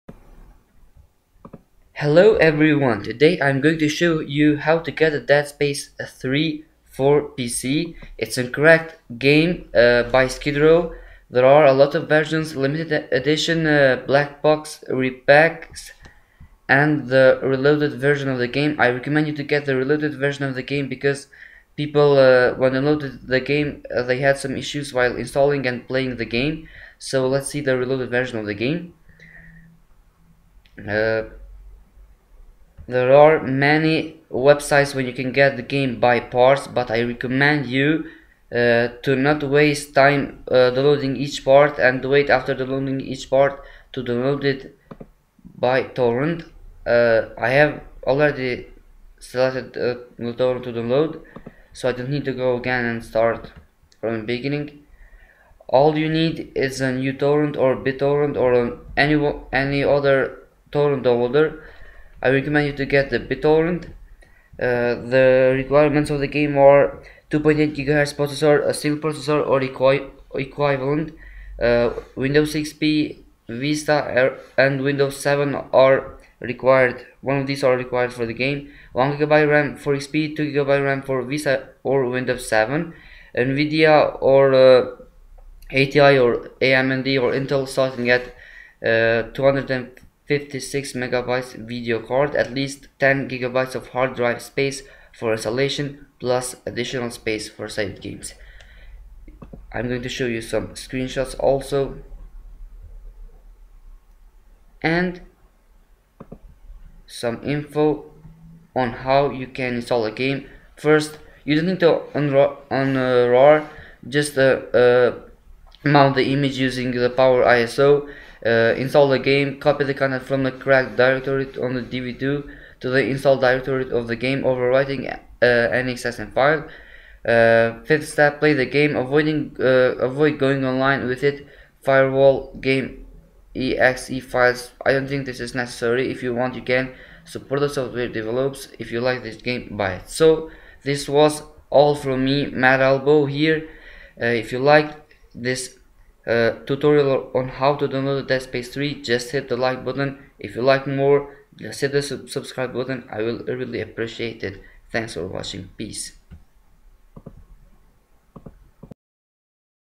How to download and Install Dead Space 3.PC
tutorial on how to download and install Dead space 3 for pc
all what u need is: